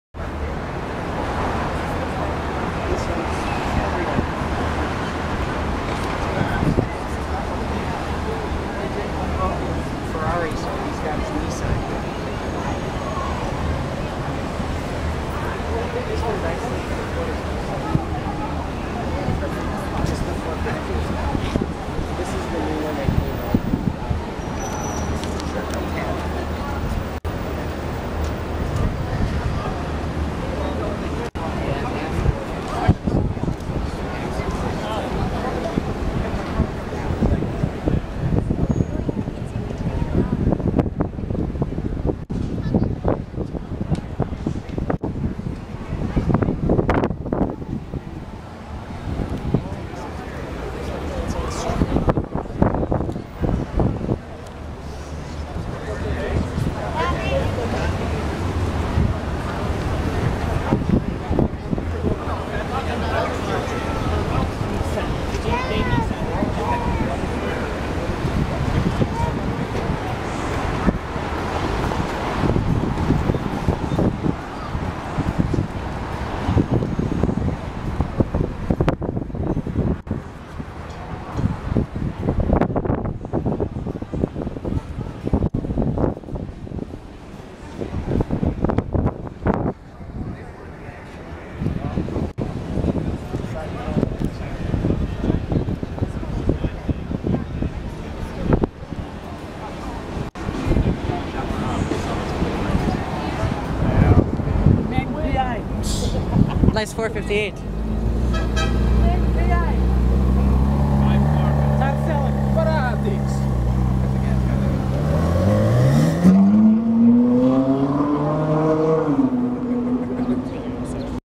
Ferrari 458 acceleration!
I went to the Ferrari Club of Central America gathering yesterday and found this exquisite Ferrari 458 Italia! Sorry about the camera wind noise and bad quality compared to handheld camcorders... I'm thinking about getting a new camera!